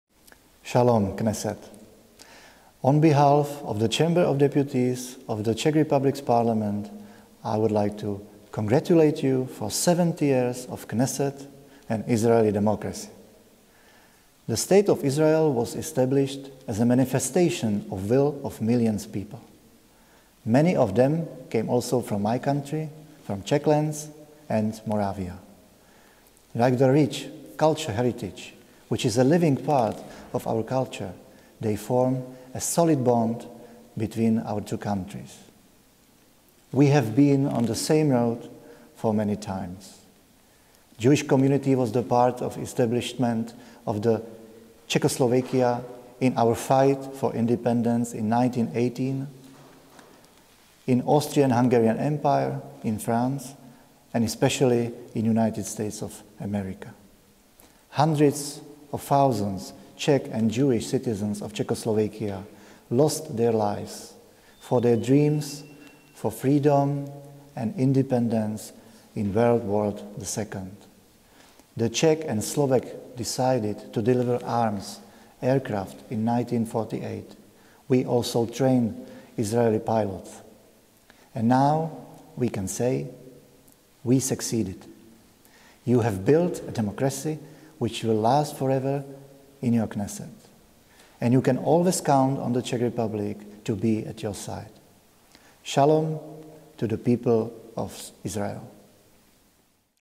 Greetings from the Chamber of Deputies of the Parliament of the Czech Republic
Greetings from Radek Vondráček, Speaker of the Chamber of Deputies of the Parliament of the Czech Republic, on the occasion of the Knesset's 70th Anniversary